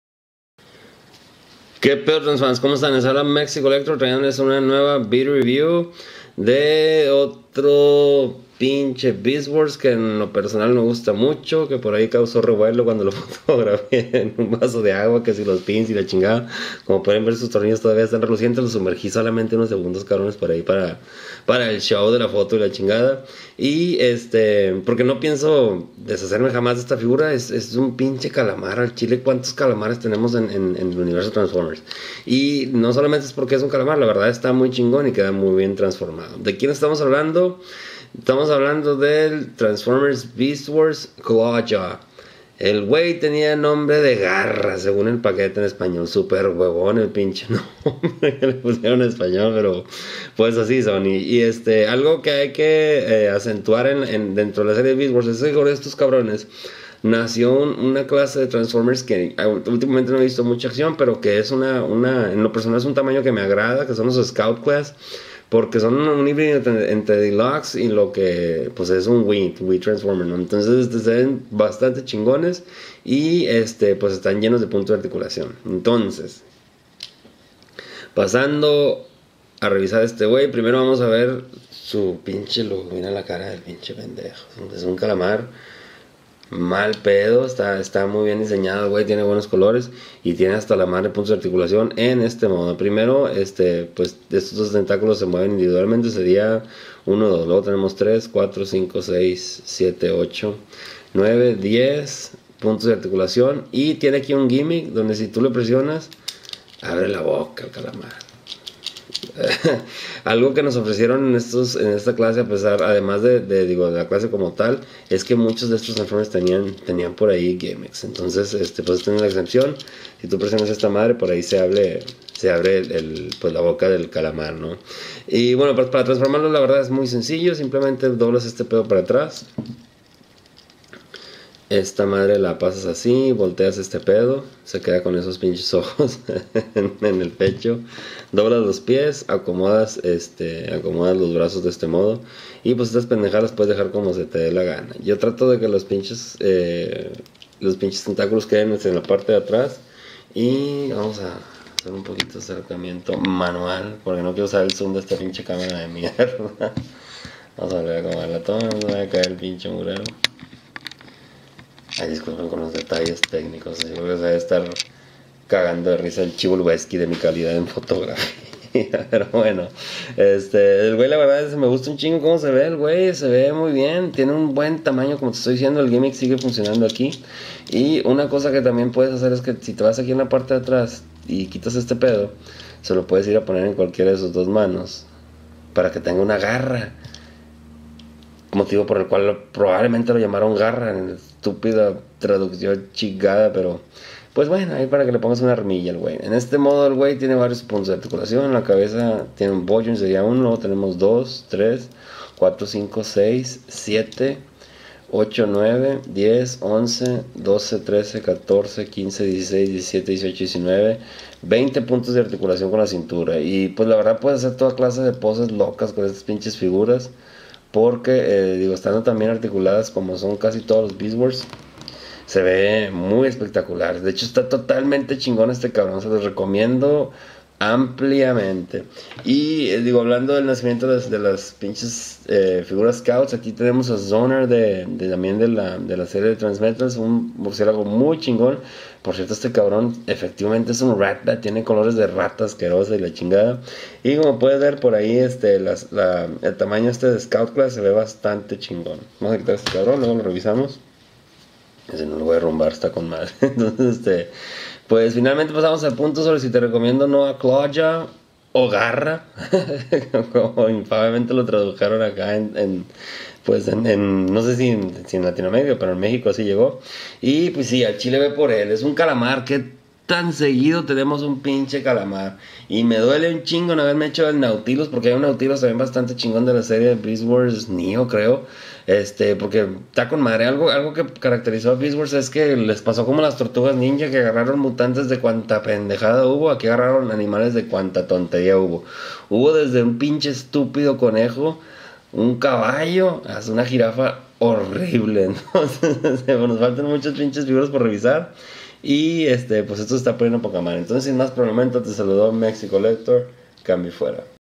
Beast Wars Claw Jaw review en español
Beast Wars nos dio la oportunidad de tener todo tipo de modos alternos como ningúna otra serie, y pues este compi es si mal no recuerdo el único calamar en toda la historia de nuestros transformers, un hecho ke por sí solo lo hace muy recomendable, pero aparte la figura es muy buena así ke revisa a este rarísimo ejemplar conmigo!

Vídeo revisión expliquitativa y crítica, haciendo uso del derecho de "uso justo"